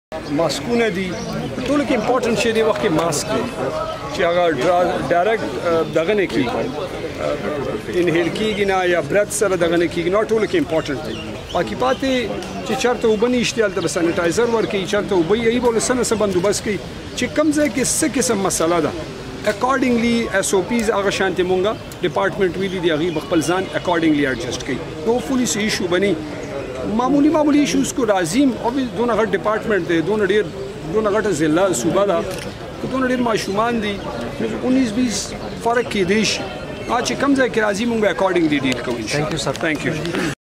Visiting Hight school Peshawar
I had a visit to the Government High School Peshawar today to observe the implementation of SOPs devised for prevention from Corona virus. We will have to adapt certain habits in this new normal.